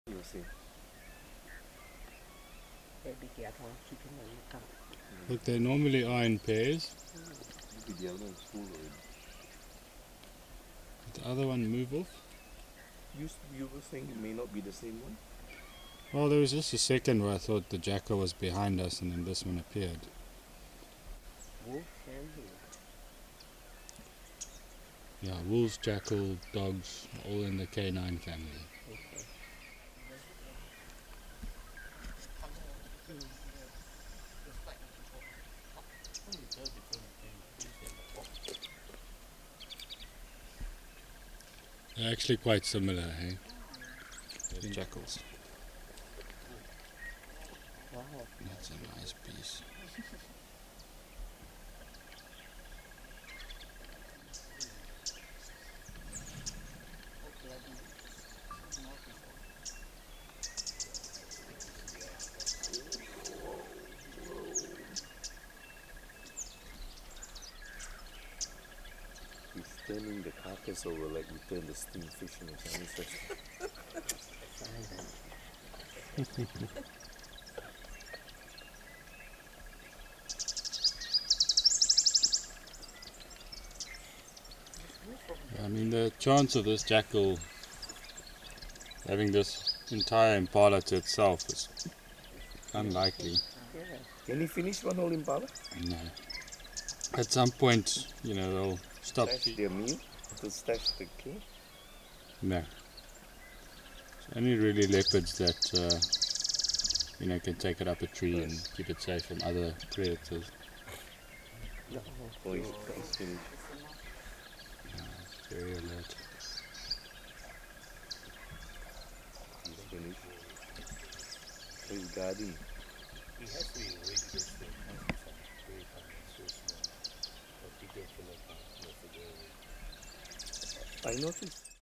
JACKAL Scores a free meal!!!
Black-backed Jackal are opportunistic scavengers and will take a free meal any day. This Impala died off a natural cause since no other predators were around. Lions and Hyenas would have eaten some of it and a Leopard would have stashed it somewhere more concealed.